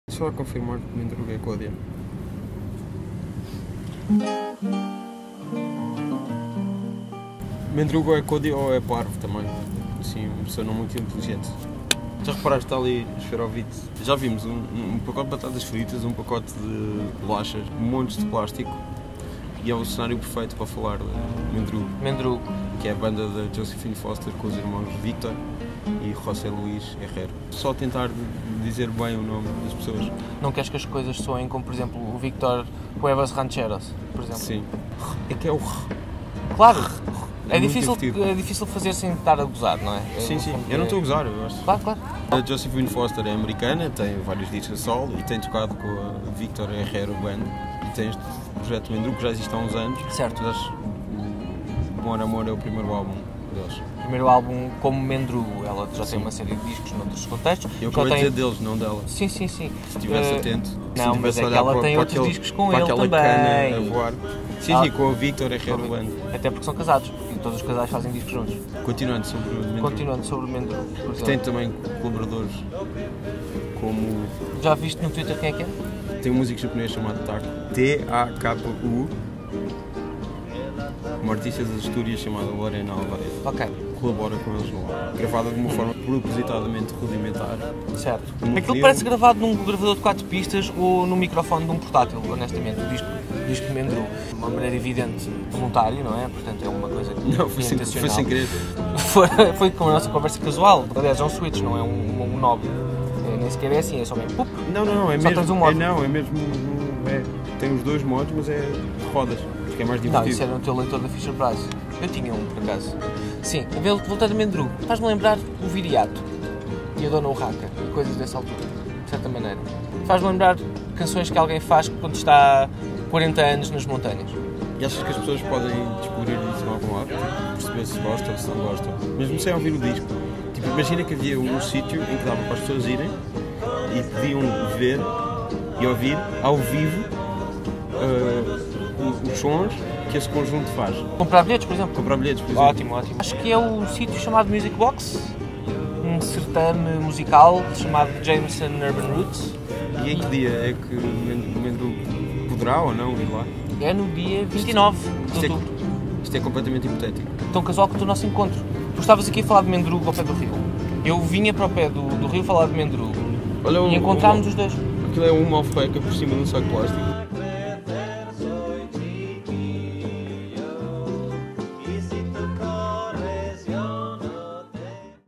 Conversa Casual #3: Mendrugo
Mendrugo é o nome do mais recente projecto de Josephine Foster e apresenta-se em Lisboa este Sábado com um disco de estreia que tem boas razões para alimentar mais uma conversa informal entre Rodrigo Nogueira e António Maria Correia.

Sábado . 29 de Outubro . 21h00

Captação, Edição e Montagem: Ana Viotti